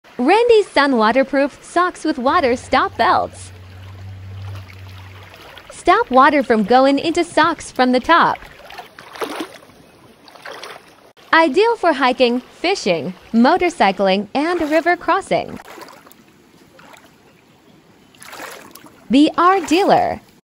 How can I stop water from going into waterproof socks from the top？
RANDY SUN breathable waterproof socks for river crossing!
We are looking for dealers all over the world!